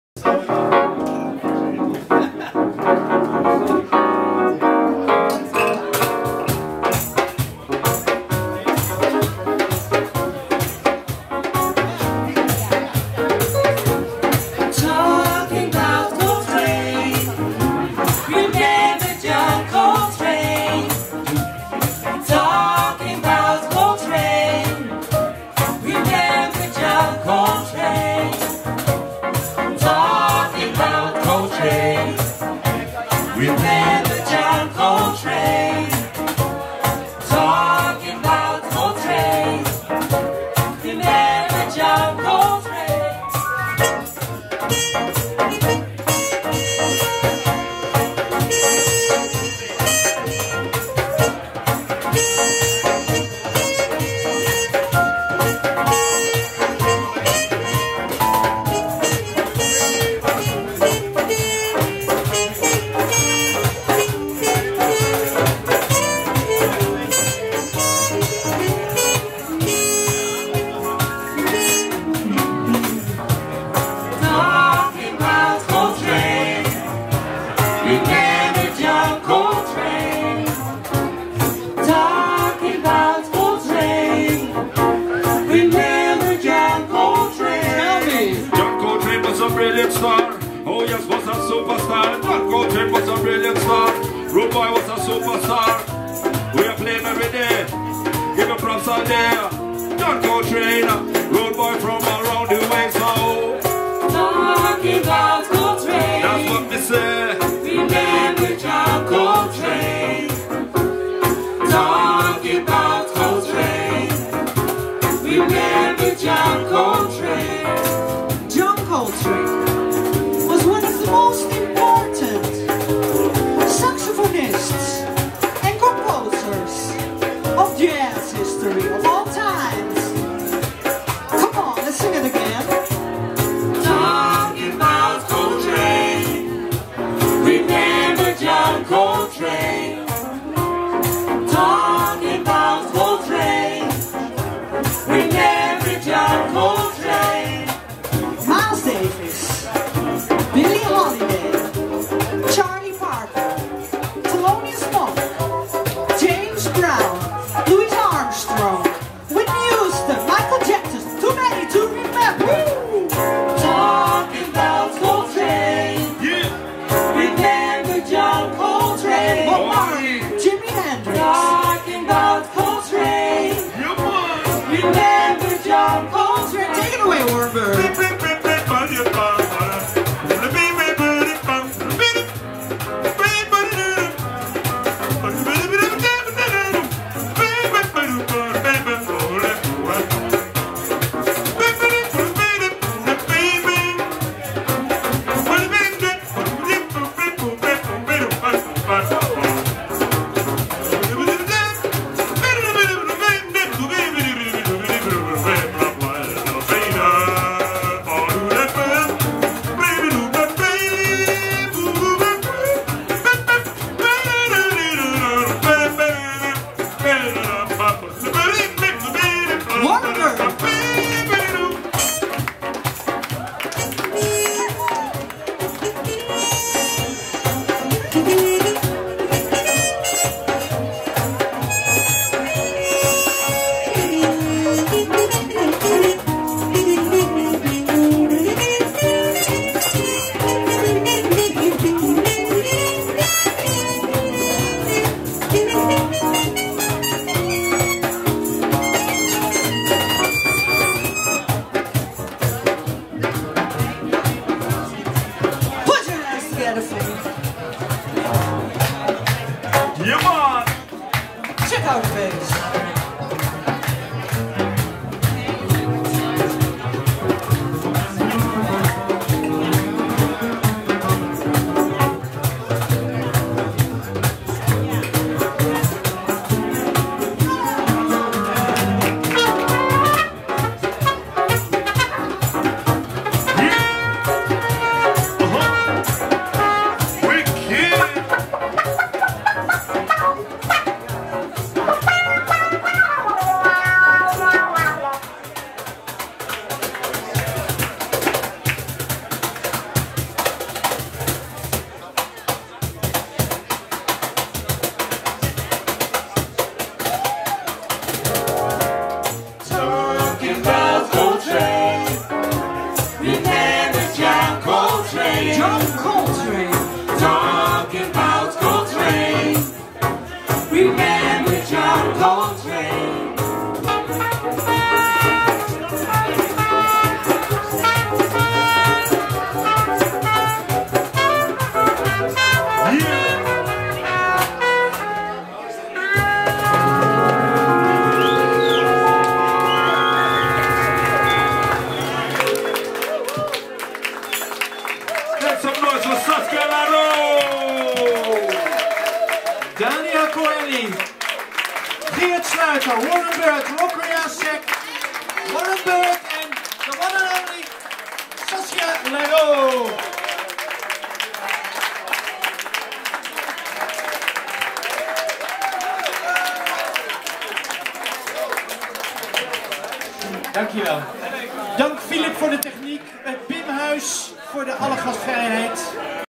Saskia Laroo Band @ World Blend Cafe - Bimhuis - Amsterdam 4
de band
Saskia Laroo - trompet/zang
Warren Byrd - piano/zang
Daniel Gueli - contrabas
Geert Sluijter - cajon
Rokhaya Seck - zang/percussion
MC Phantom - rap/zang